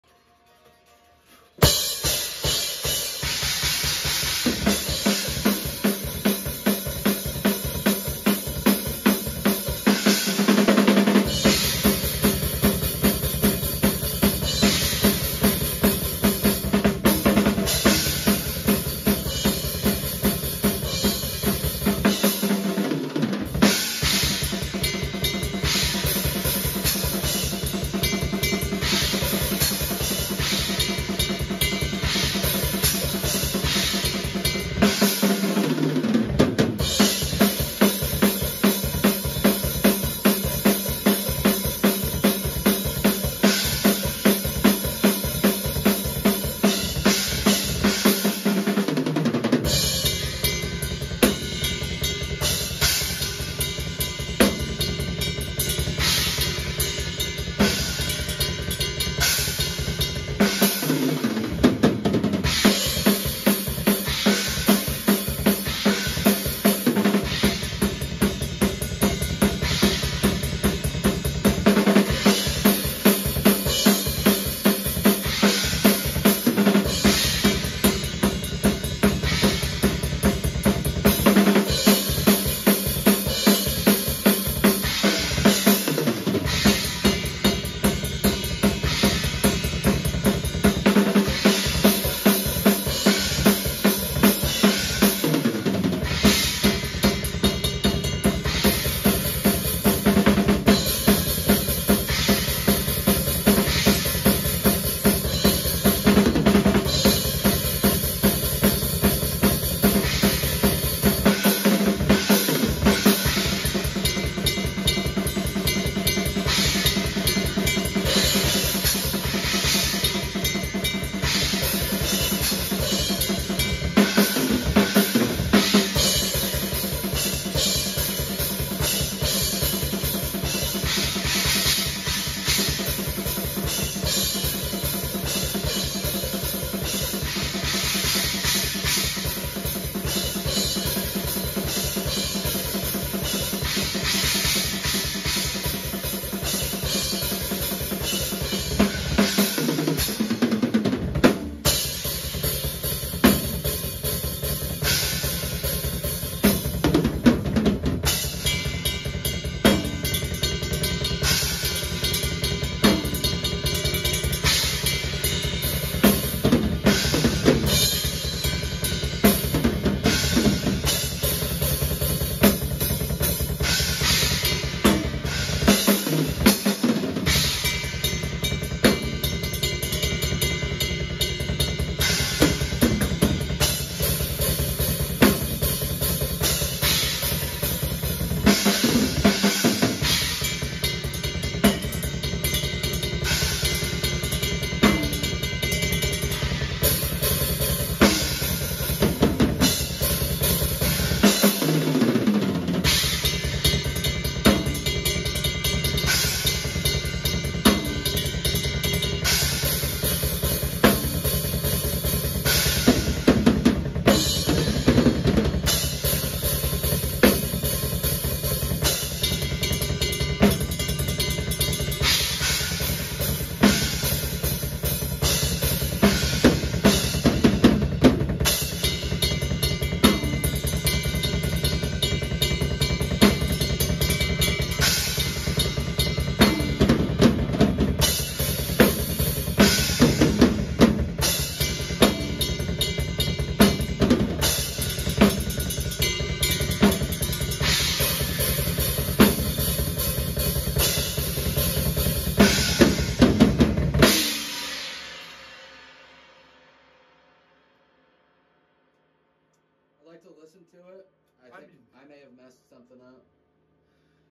Cataphract Drum Demo - Nolan Weber - Kansas City Black Metal
Recording a demo of a newer Cataphract song sometime last year, although this take and session was not used. I figured I'd put it up since it doesn't sound bad and looks kinda cool. All Paiste, all Remo, mostly Tama (besides my PAWG snare), and ACD Unlimited pedals!